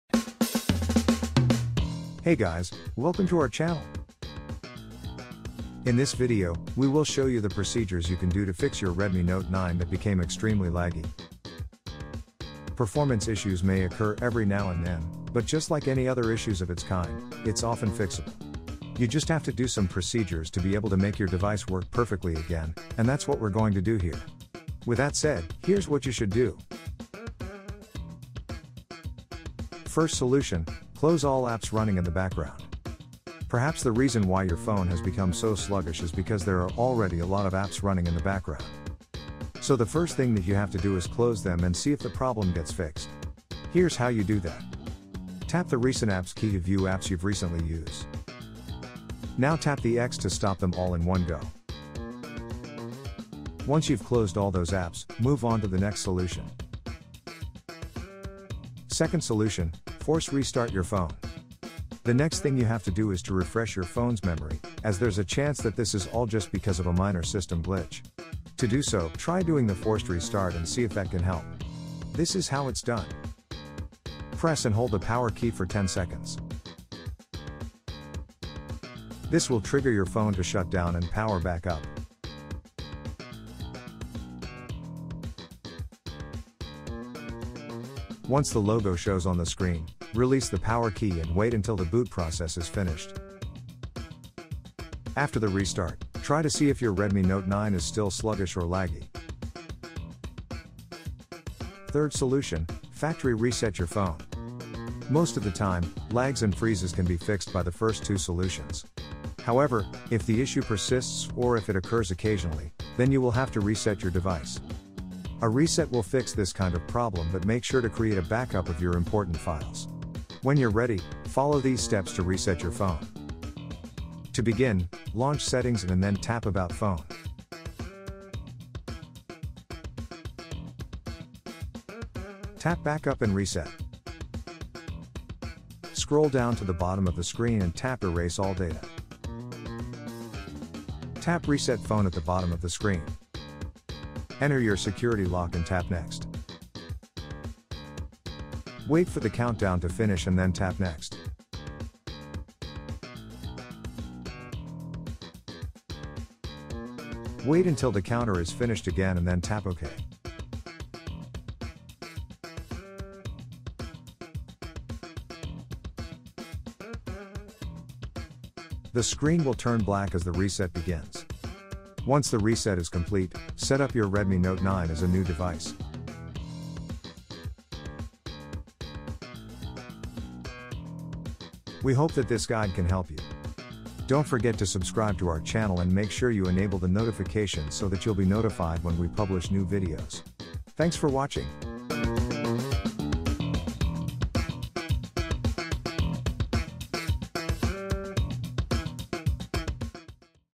How To Fix A Redmi Note 9 That’s Extremely Laggy
Hey guys, welcome to Pinoy Tech Tips!

In this video, we will show you the procedures you can do to fix your Redmi Note 9 that became extremely laggy.

Performance issues may occur every now and then, but just like any other issues of its kind, it’s often fixable. You just have to do some procedures to be able to make your device work perfectly again, and that’s what we’re going to do here. With that said, here’s what you should do:

First solution: Close all apps running in the background
Second solution: Force restart your phone
Third solution: Factory reset your phone